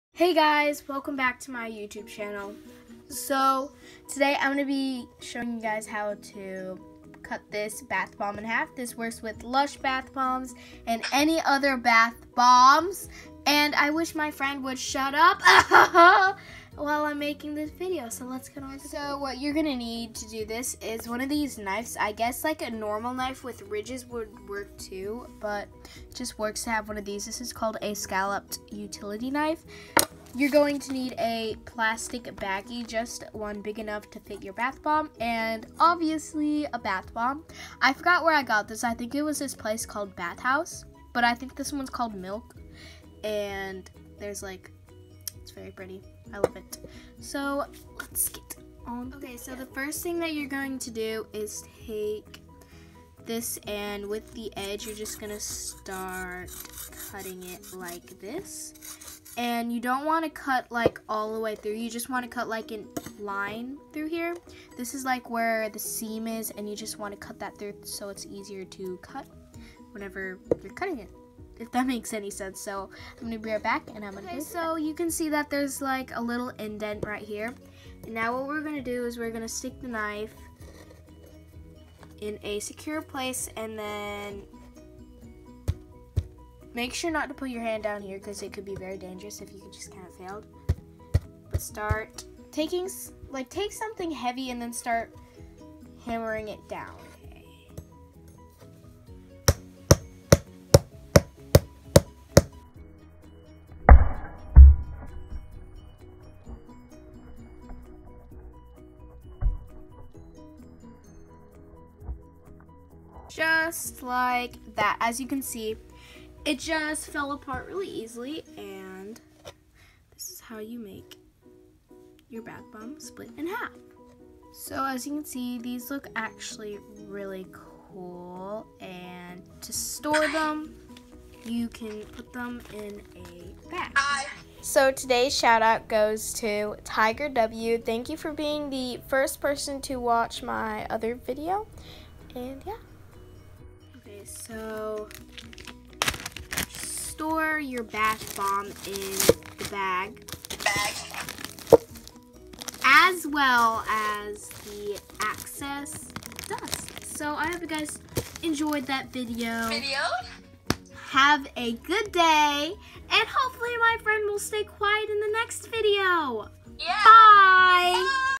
How To Split Up A Bath bomb // My Friend Won’t Stop Talking //
Hey guys! I hope you enjoyed this video and I also hope that you also learned some stuff from it! My friend was talking during it since we were FaceTiming 😂😂😂